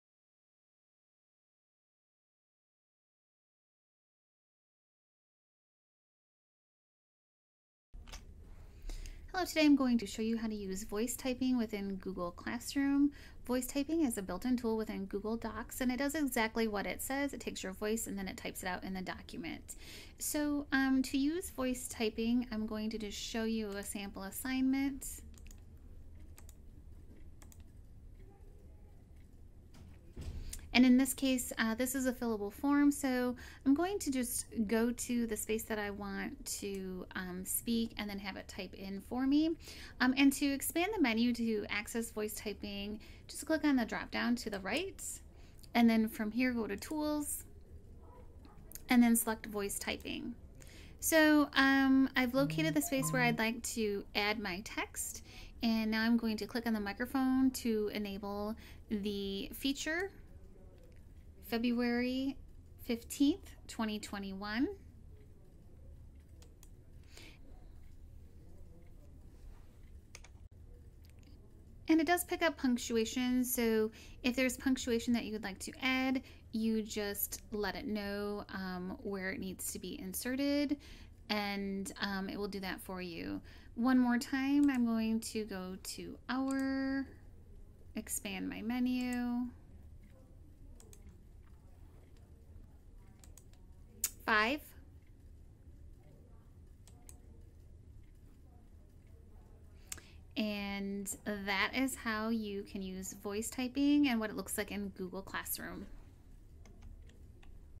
Google Classroom - Voice Typing
How to use Voice Typing in Google Classroom